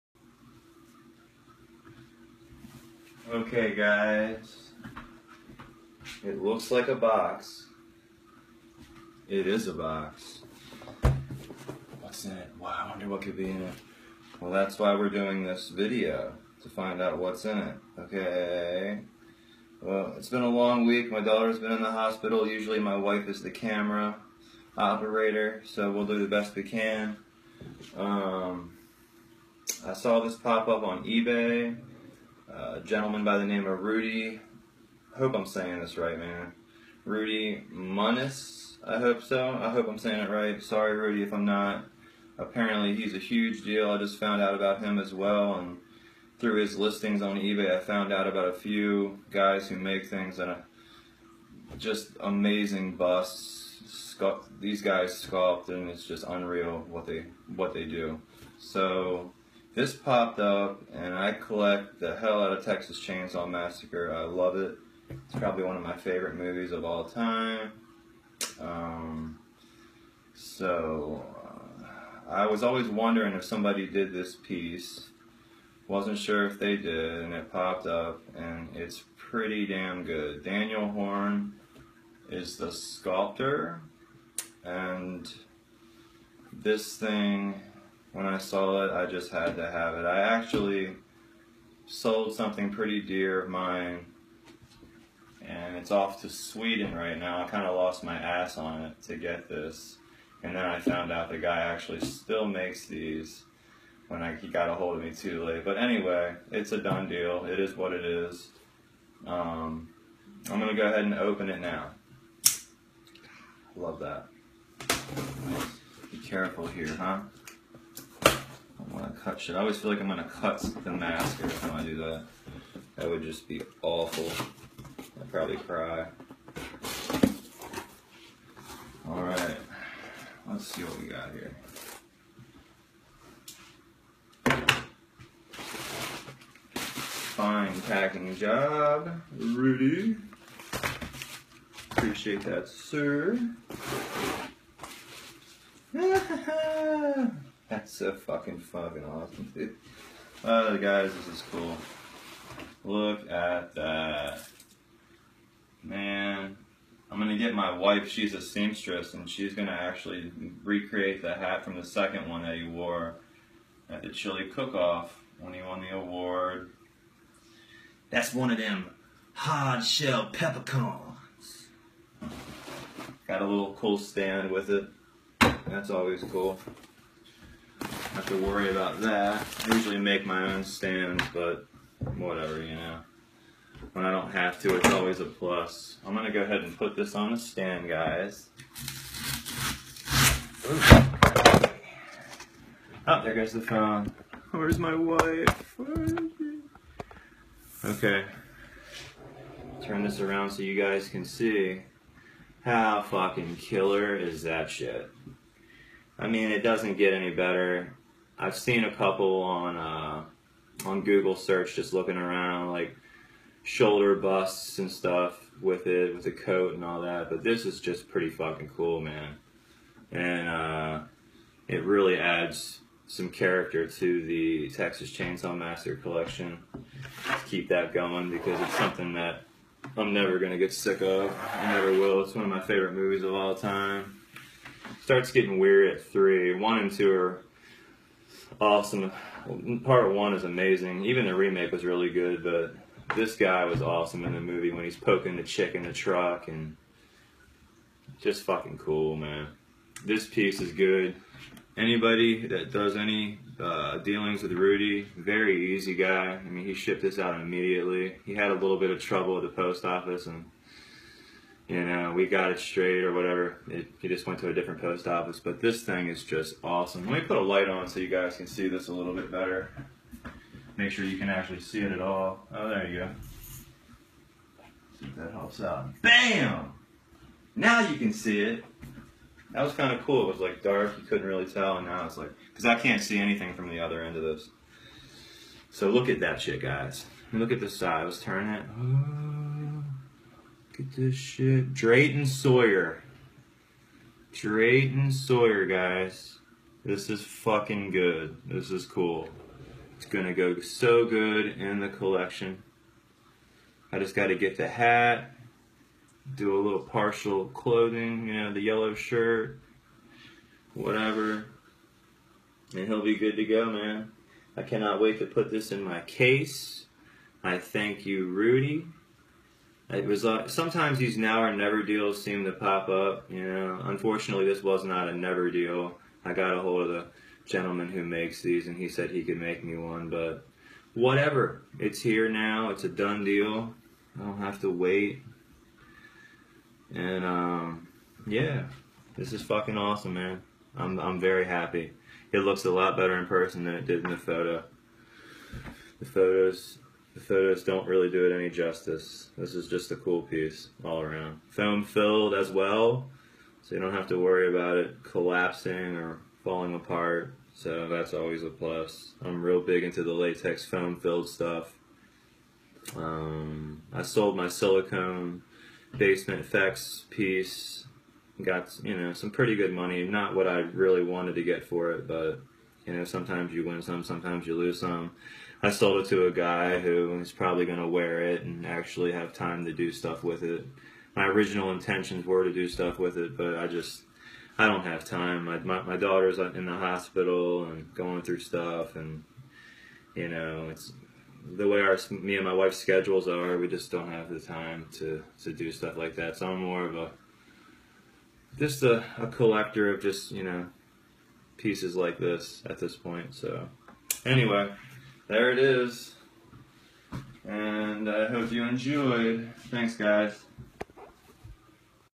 Drayton Sawyer from TCM part 1 and 2 latex Bust
This is a Daniel Horne sculpt. Daniel is one of the most talented sculptors/painters i know. An unbelievable talent.This is his take on the cook from one of my favorite series of all time. Enjoy my wife added ribbon to a hat we bought on ebay to make it resemble the one he wore in TCM part 2